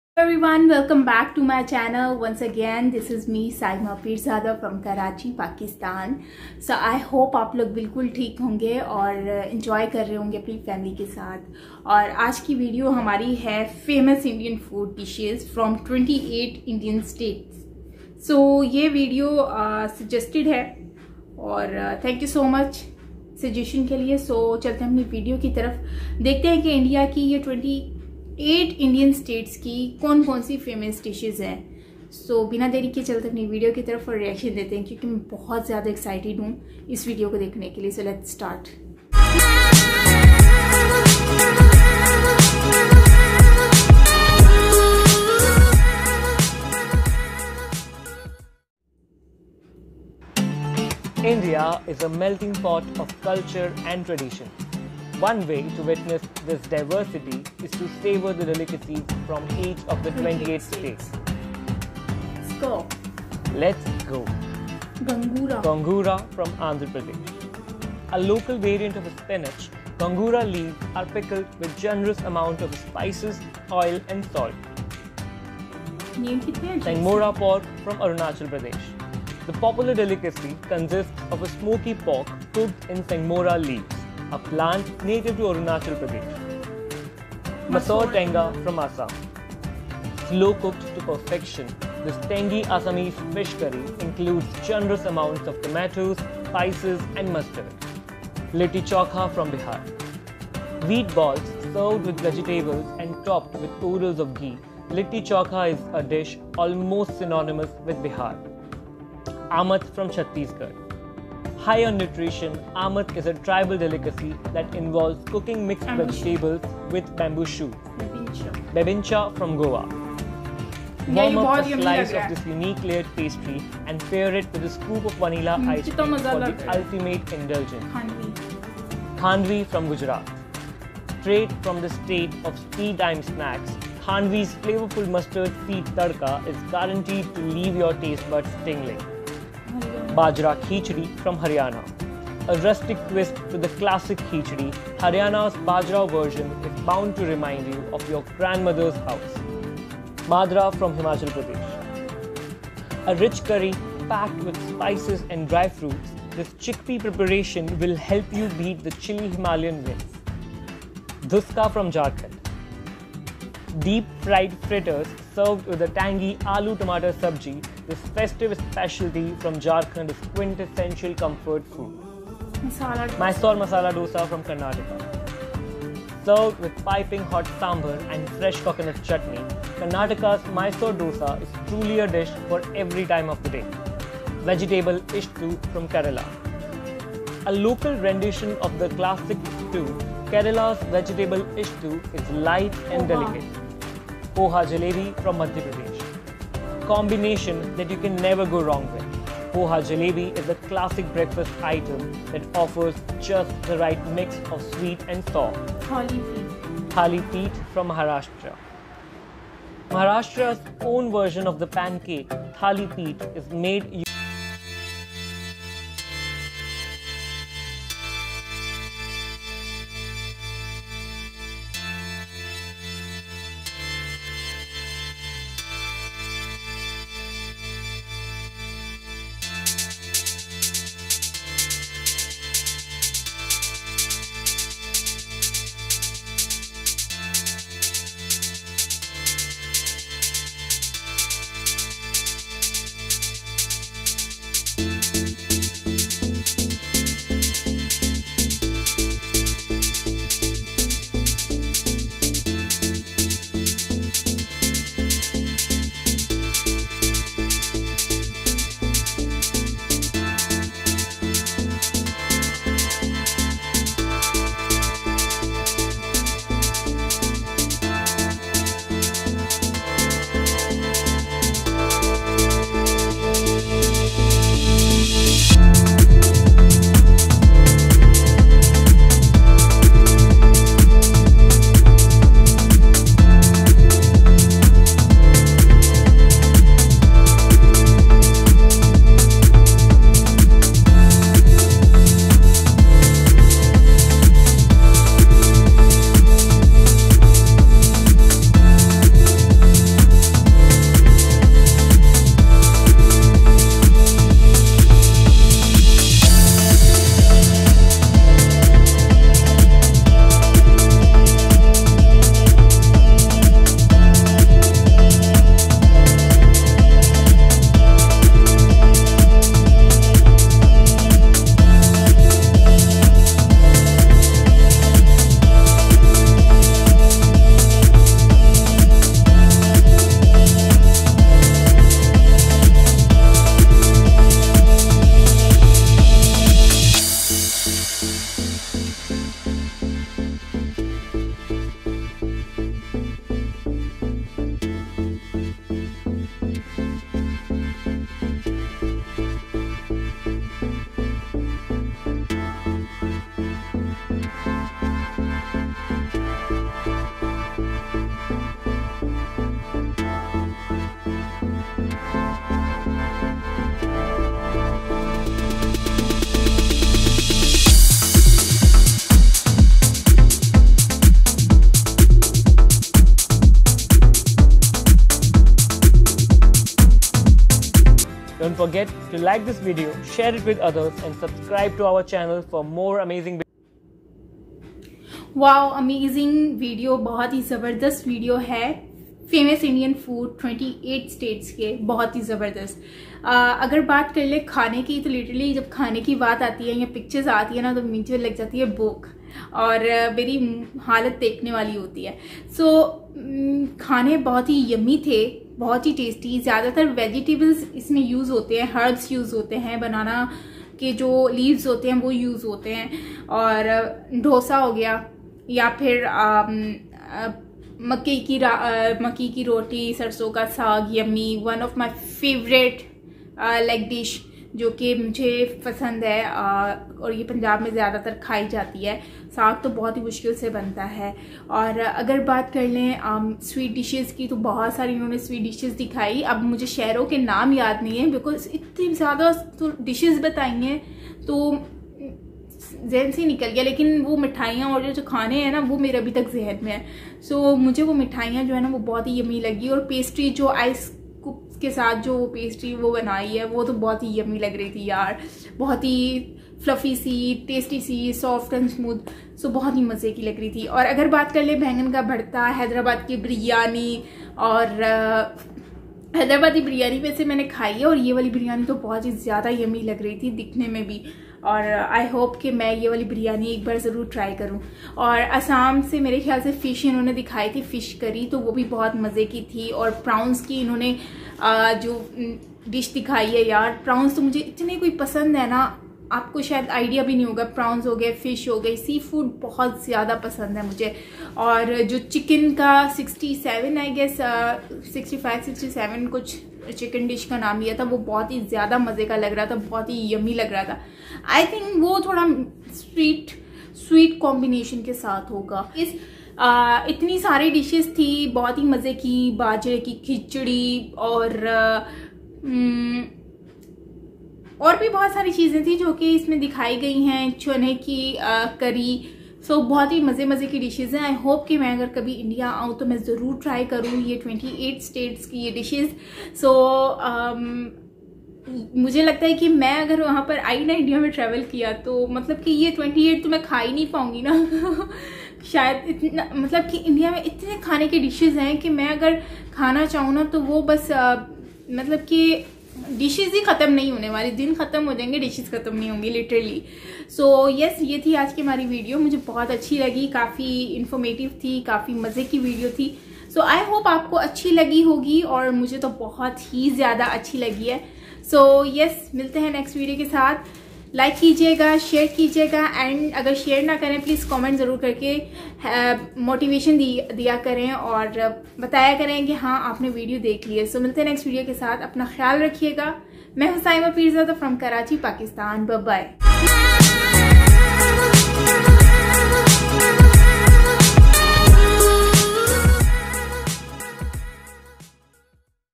reaction | FAMOUS Indian Food Dishes From 28 Indian States | Indian Cuisine| saima pirzada
Indian Food Sarno ka saag makai ki roti one of my favorite Dish and hyederabadi biryani too😋 whats your ❤

I am using this video for giving my feedback only I am not using anything or music from this video basically I am giving my fair reaction & I am not owning anything from this video.